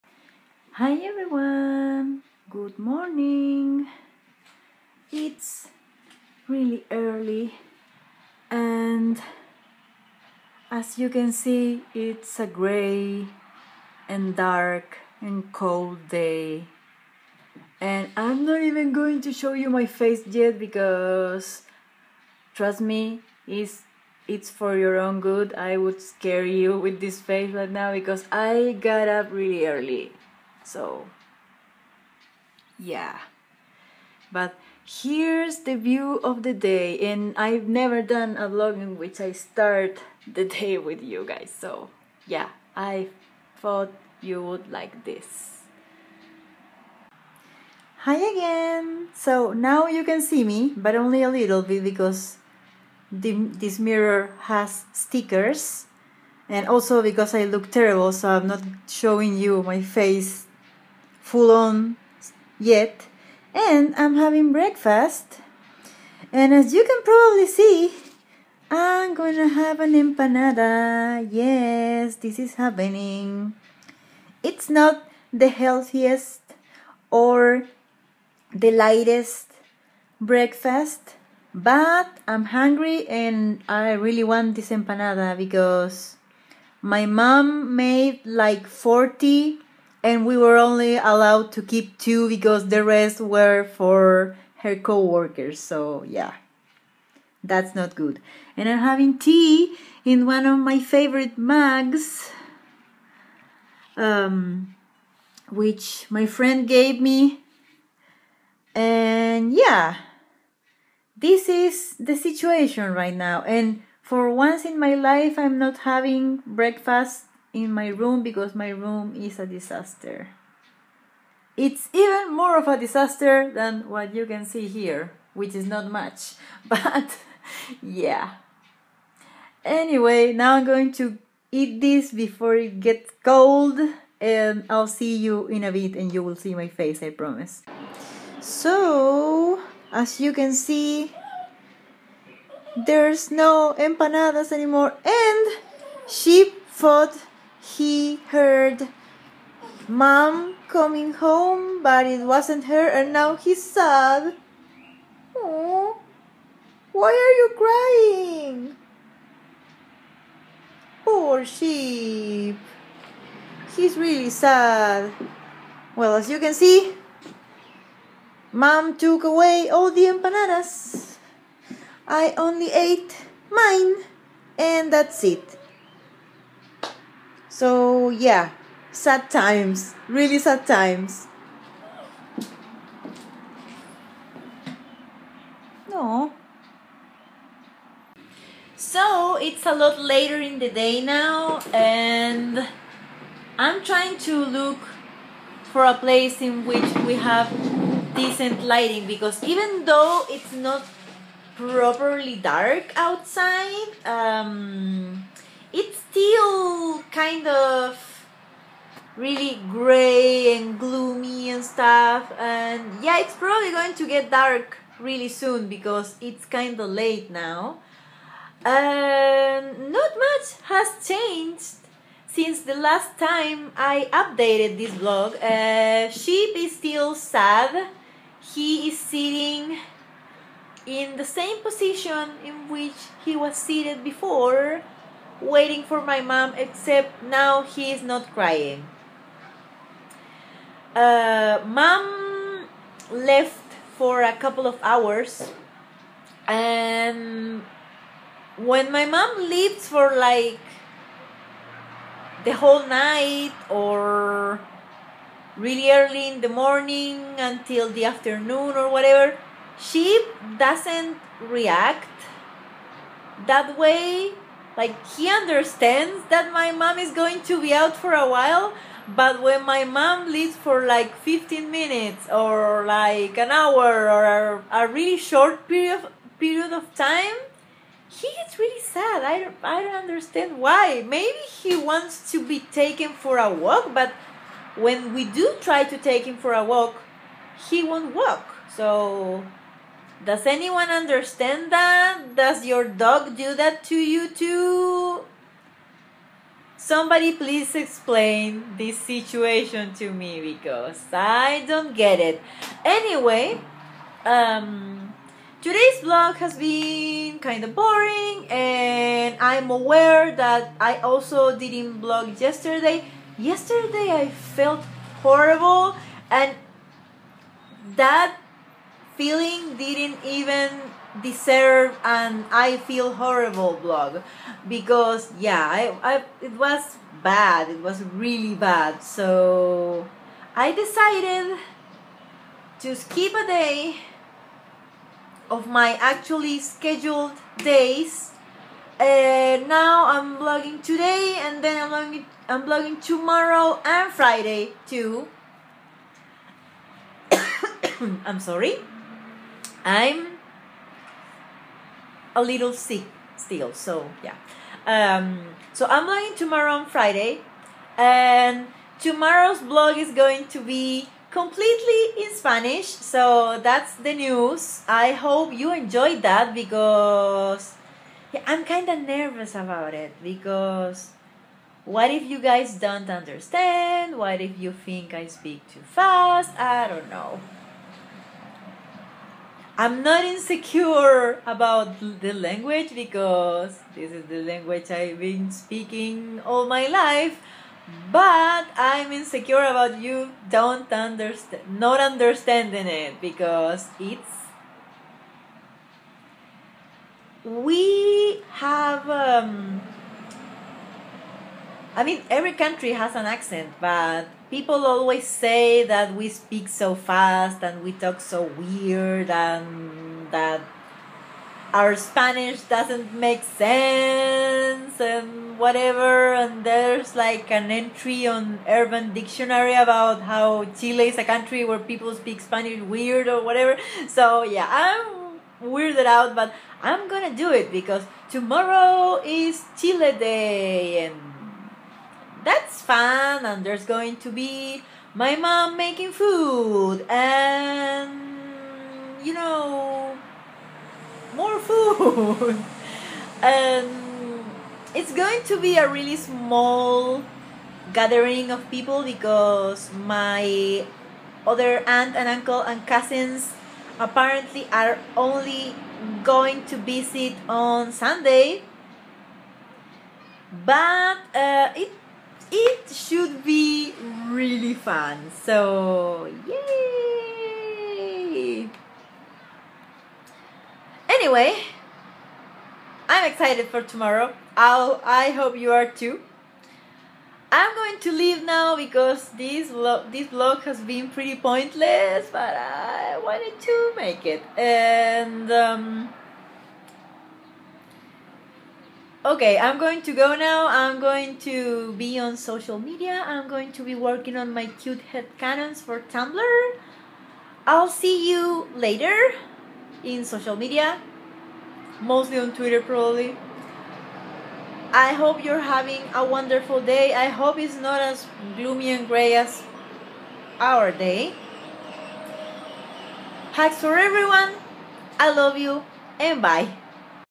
Food, Sad Sheep & Gloomy Day - September Vlog #10
It was a chill day before the upcoming holidays :)